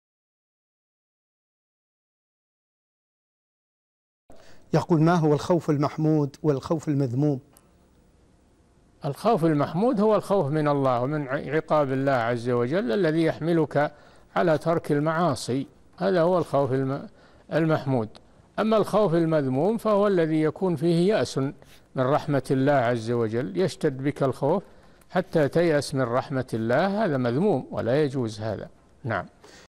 1406 - الخوف من الله .. متى يكون محموداً ومتى يكون مذموماً؟ - الشيخ صالح الفوزان
"نص السؤال: الخوف من الله .. متى يكون محموداً ومتى يكون مذموماً؟
السيرة الذاتية :
للاشتراك في قناة الشيخ صالح الفوزان
لمشاهدة حلقات فتاوى على الهواء كاملة

للاستفسار والإقتراحات والدعم
رئيس الفريق
لمشروع كبار العلماء